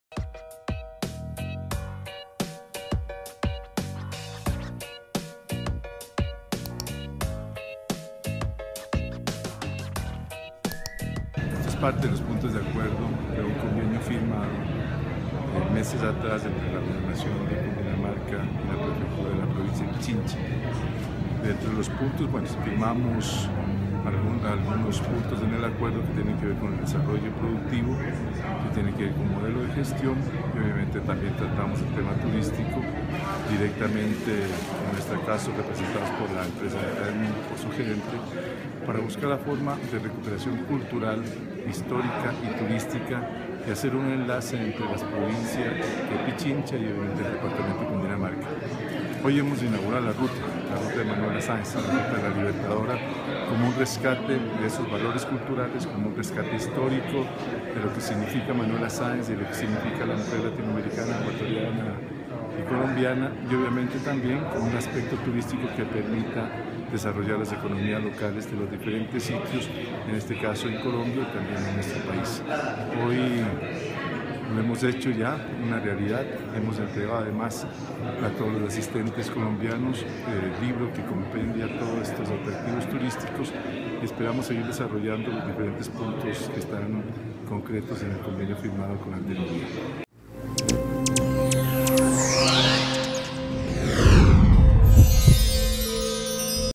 Entrevista al Prefecto de la Provincia de Pichincha, Econ. Gustavo Baroja.
El Prefecto de la Provincia de Pichincha, Gustavo Baroja, visitó la Mina de Sal de Zipaquirá, destino incluido en la Ruta Turística "Manuela La Libertadora" Pichincha-Cundinamarca, donde se realizó su lanzamiento.
El Sr. Prefecto señaló la importancia del Acuerdo de Cooperación Binacional para el desarrollo de las ciudades incluidas en la ruta turística, misma que también rescata el patrimonio de los pueblos hermanos, y resalta la importancia de mujeres admirables como Manuela Sáenz y Policarpa Salavarrieta.